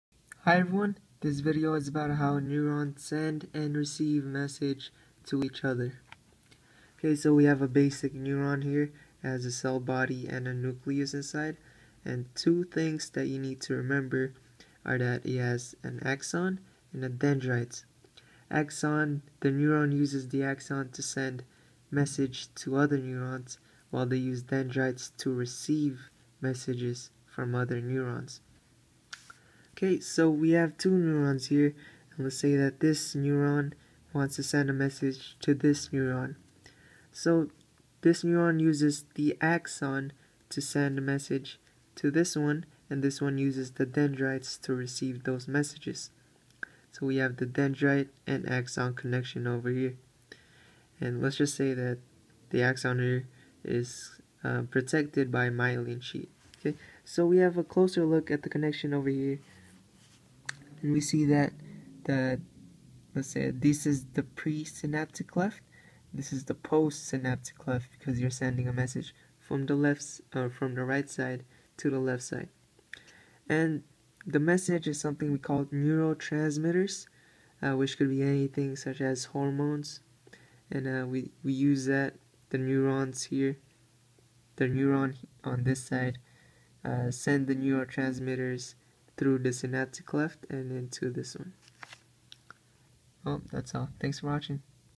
How Neurons Send Messages
SUMMARY
Neurons have axons and dendrites. Dendrites receive impulses and axons sends impulses from the neurons. In this example, the right neuron wants to send a message to the left neuron - it does so by sending the message (neurotransmitters) through the axons and into the left neuron's dendrites. The interaction, taken a closer look, is made by the presynaptic cleft sending an impulse through the synapse and into the postsynaptic cleft.